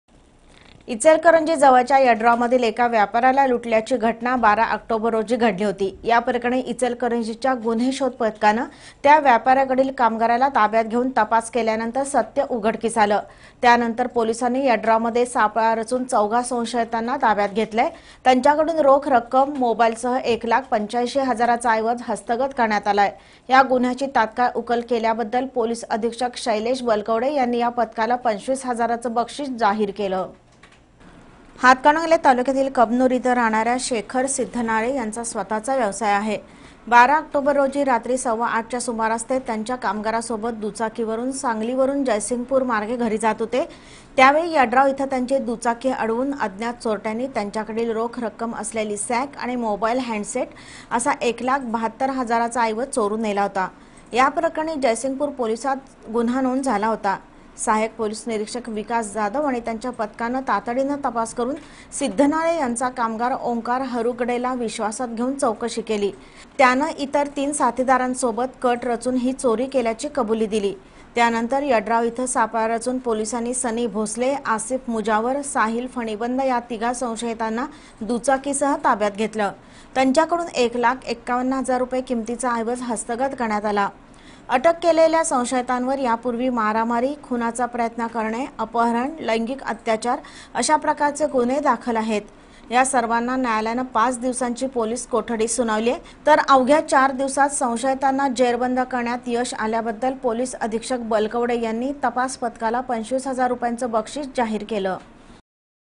इचलकरंजी जवळच्या यड्राव इथल्या व्यापार्‍याला लुटल्या प्रकरणी गुन्हे शोध पथकानं केली चौघांना अटक
इचलकरंजी जवळच्या यड्राव इथल्या व्यापार्‍याला लुटल्या प्रकरणी गुन्हे शोध पथकानं केली चौघांना अटक, रोख रक्कम आणि मोबाईलसह १ लाख ८५ हजाराचा ऐवज हस्तगत
Channel B